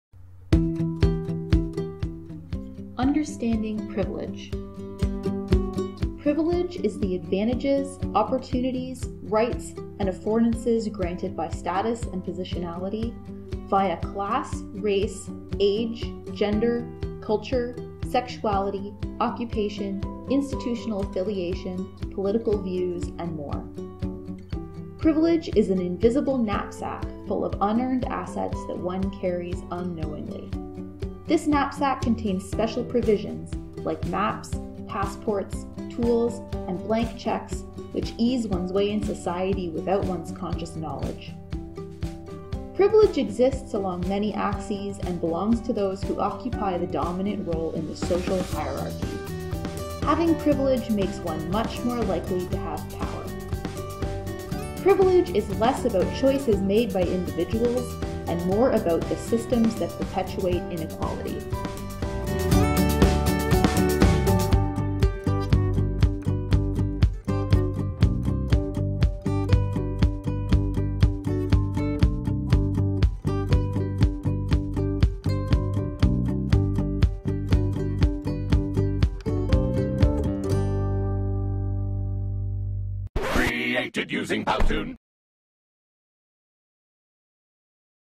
Understanding Privilege
Sources

Definition of privilege:
Booth, C. (2014, December 1). On information privilege [Blog post].

Knapsack metaphor:
McIntosh, P. (1989, July). White privilege: unpacking the invisible knapsack. Peace and Freedom Magazine, 10-12.

Music byJosh Molen  (TheTunePeddler.com)